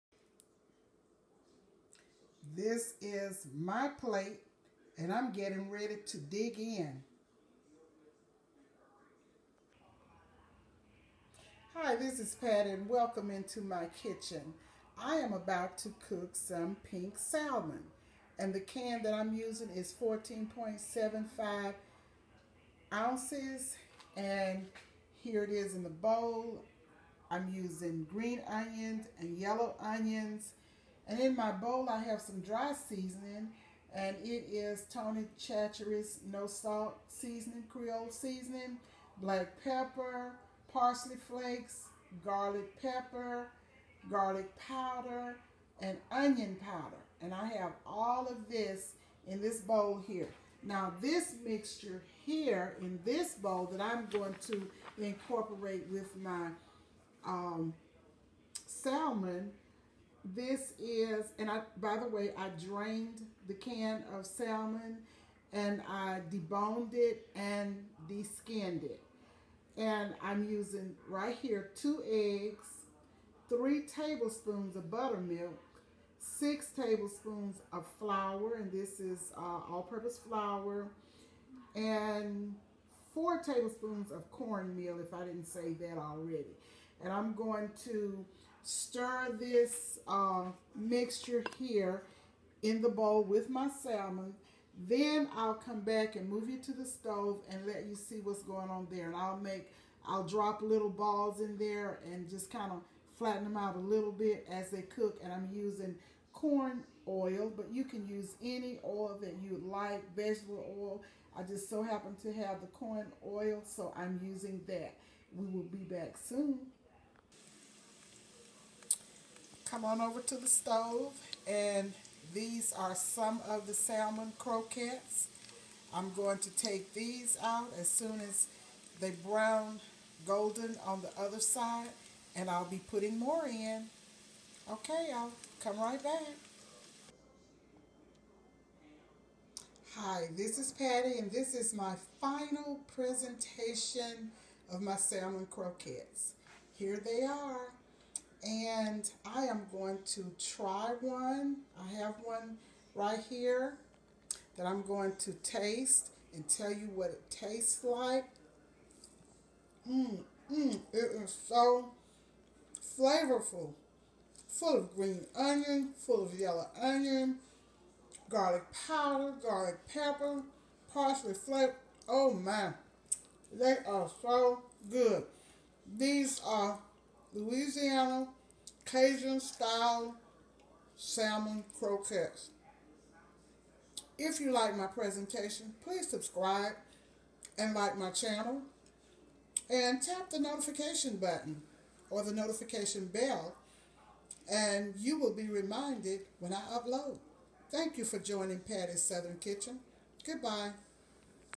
How to Make: Salmon Croquettes & Rice 🍚 Recipe is in Description box.
SALMON CROQUETTES

14.75 oz can Salmon
Medium yellow onion, 2 stalks green onions, salt, pepper, dash Creole seasoning- all to your liking.
2 eggs
3 Tbsp buttermilk
4 Tbsp cornmeal
6 Tbsp all purpose flour
Vegetable oil to fry or oil of your choice.

1.  Combine Salmon, dry seasonings, onions.
2.  Stir in eggs, buttermilk, cornmeal & flour. Combine well.
3.  Heat 2 to 3 Tbsp oil in skillet (I used case iron skillet), medium high heat. Add more oil as needed.
4. Make balls and pat them out a little once they are in the skillet.
5.  Fry on both side until brown.

Deliciously crispy on the outside and amazingly moist and flavorful on the inside.

Delectable!😋